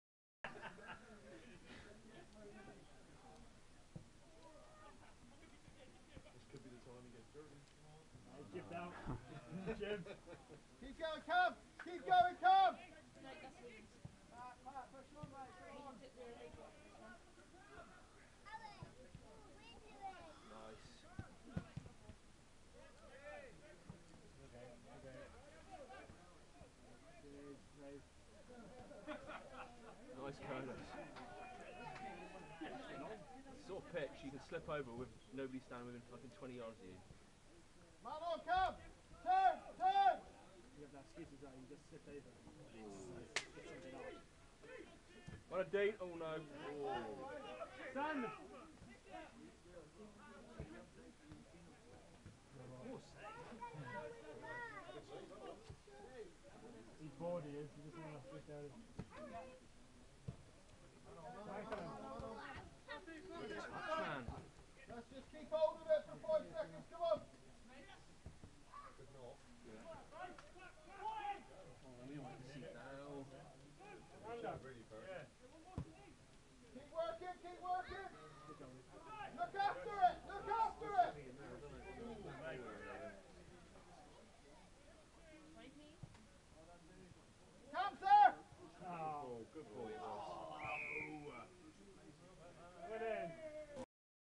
C.C,F.A Bond Timber Junior Cup 3RD Round Game, v St Stephen 10 v 0 Halsetown
match highlights Courtney Rowe passes to his younger brother who scores but the goal is ruled out for offside.